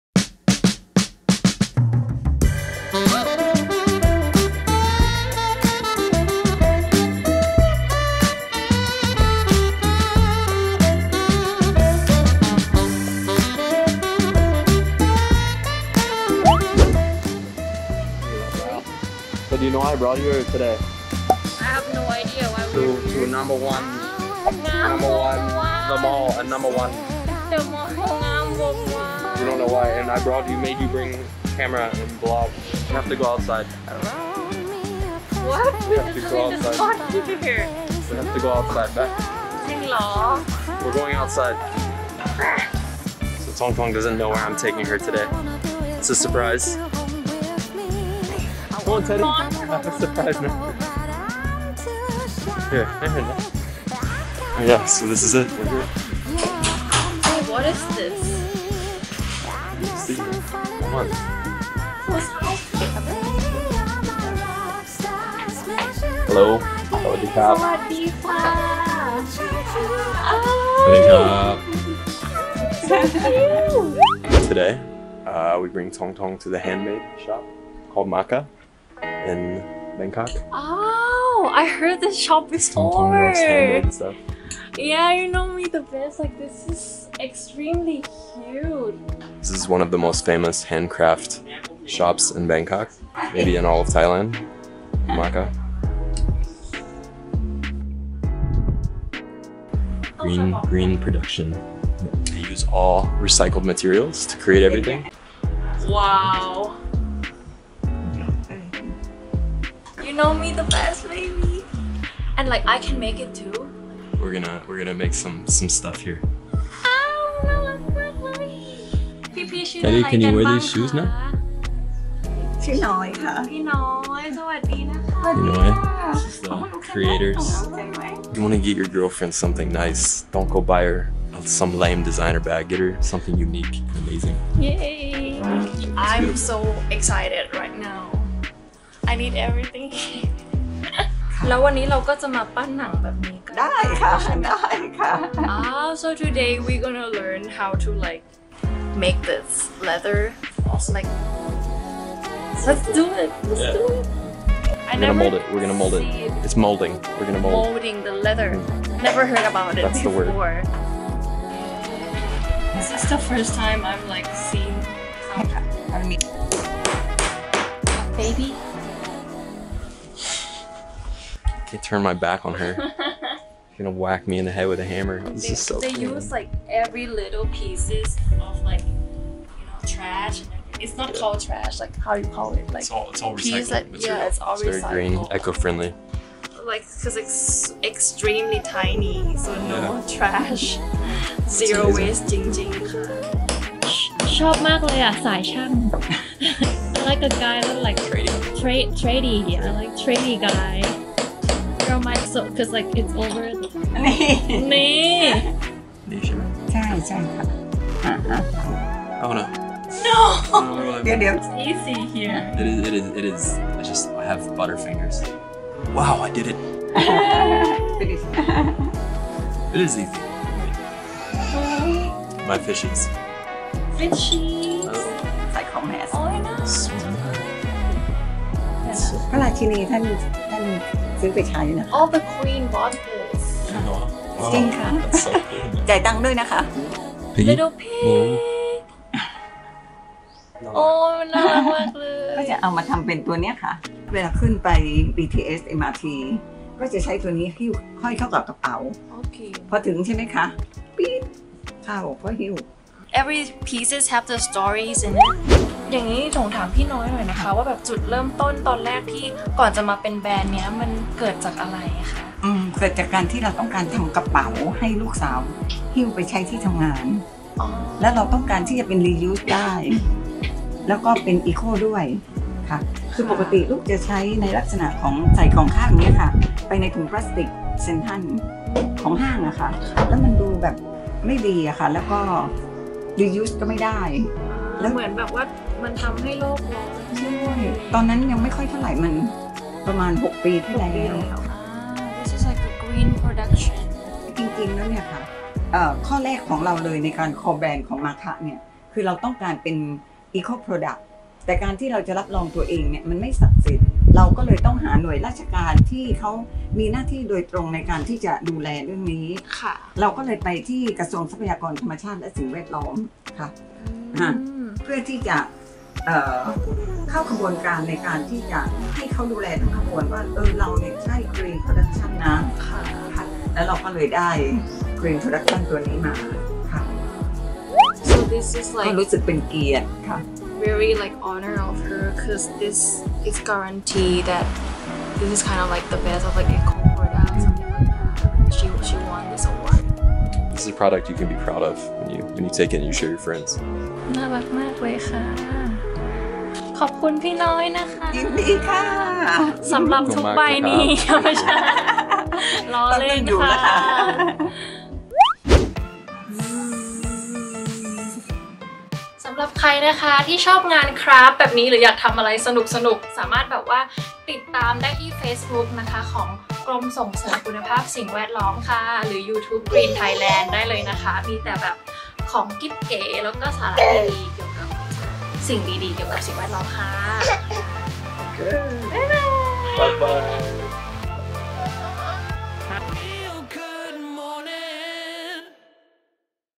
มาฆะดีไซน์ EP.5
แนวคิดสินค้าแฮนเมด งานดีไซน์รักษ์โลก ที่มีจุดเริ่มต้นจากความรัก
ร้านมาฆะดีไซน์  ร้านที่ได้รับรางวัล Green Production ระดับประเทศ